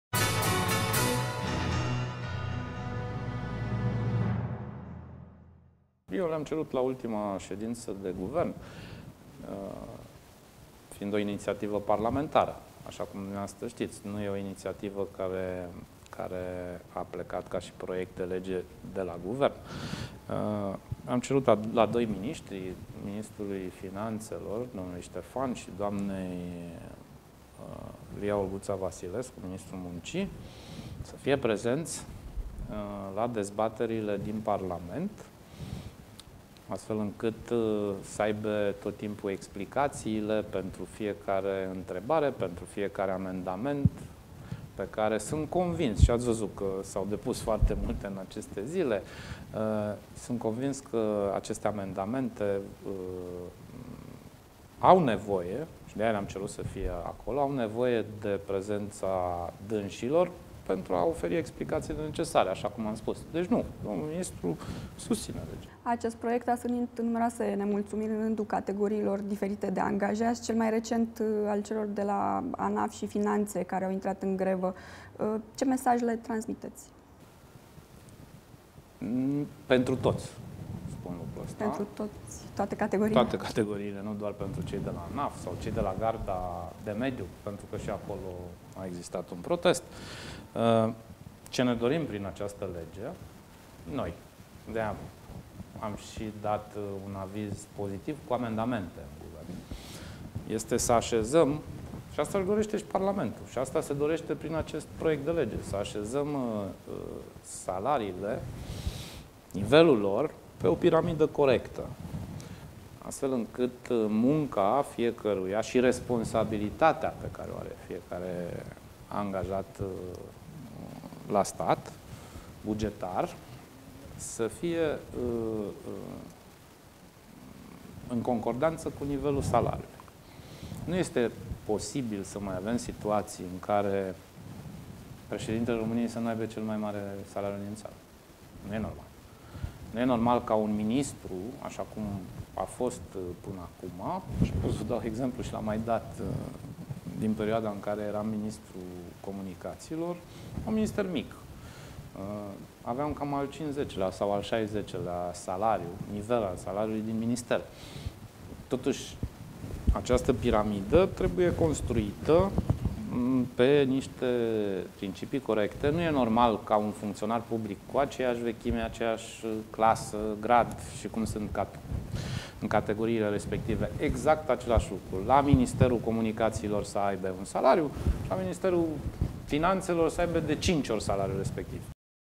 Premierul Sorin Grindeanu, despre nemulţumirile privind legea de salarizare
"Ce ne dorim prin această lege noi, de aceea am dat şi un aviz pozitiv cu amendamente, este să aşezăm - şi asta doreşte şi Parlamentul şi asta se doreşte prin acest proiect de lege - să aşezăm salariile, nivelul lor, pe o piramidă corectă, astfel încât munca fiecăruia şi responsabilitatea pe care o are fiecare angajat la stat, bugetar, să fie în concordanţă cu nivelul salariului", a declarat premierul Sorin Grindeanu, într-un interviu acordat miercuri AGERPRES.